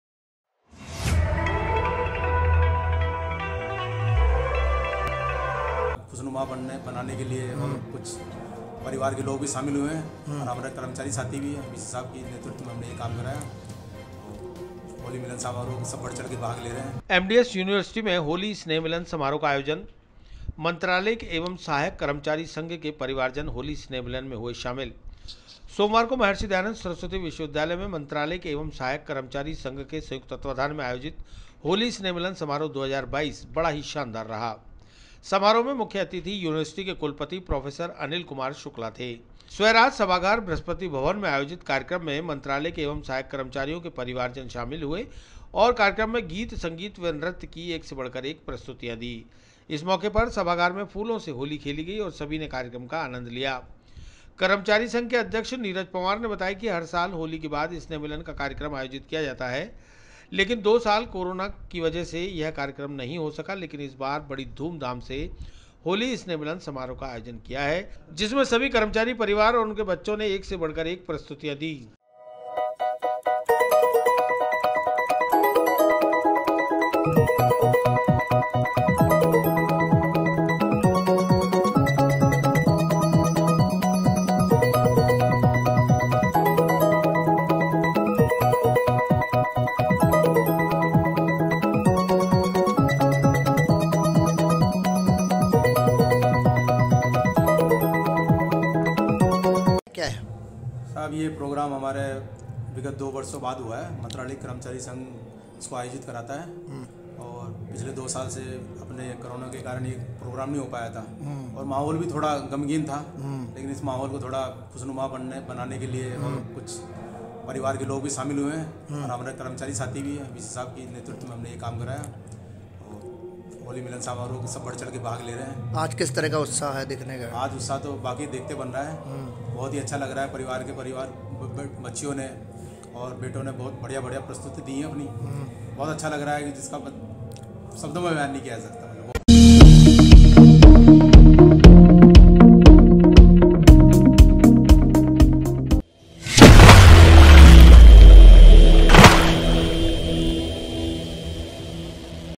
एमडीएस यूनिवर्सिटी में होली स्नेह मिलन समारोह का आयोजन
मंत्रालयिक एवं सहायक कर्मचारी संघ के परिवारजन होली स्नेह मिलन में हुए शामिल

सोमवार को महर्षि दयानंद सरस्वती विश्वविद्यालय में मंत्रालयिक एवं सहायक कर्मचारी संघ के संयुक्त तत्वाधान में आयोजित होली स्नेह मिलन समारोह 2022 बड़ा ही शानदार रहा। समारोह में मुख्य अतिथि यूनिवर्सिटी के कुलपति प्रोफेसर अनिल कुमार शुक्ला थे। स्वराज सभागार बृहस्पति भवन में आयोजित कार्यक्रम में मंत्रालयिक एवं सहायक कर्मचारियों के परिवार जन शामिल हुए और कार्यक्रम में गीत संगीत व नृत्य की प्रस्तुतियां दी। इस मौके पर सभागार में फूलों से होली खेली गई और सभी ने कार्यक्रम का आनंद लिया। कर्मचारी संघ के अध्यक्ष नीरज पंवार ने बताया कि हर साल होली के बाद स्नेह मिलन कार्यक्रम आयोजित किया जाता है, लेकिन 2 साल कोरोना की वजह से यह कार्यक्रम नहीं हो सका लेकिन इस बार बड़ी धूम धाम से होली स्नेह मिलन समारोह का आयोजन किया है। जिसमें सभी कर्मचारी परिवार और उनके बच्चों ने एक से बढ़कर एक प्रस्तुतियां दी है।